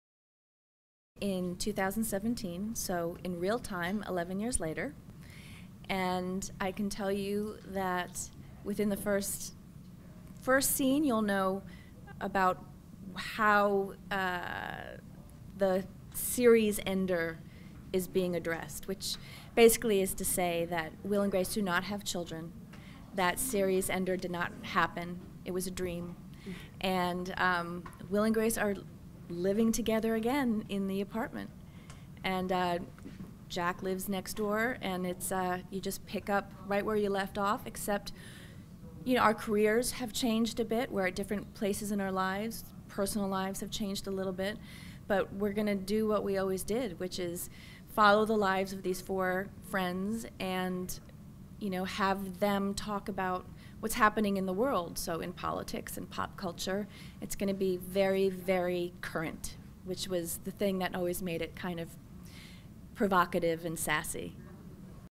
MiNDFOOD Finds Out The Truth About The New Will & Grace Series with Debra Messing
Find out what happens on the new series of Will & Grace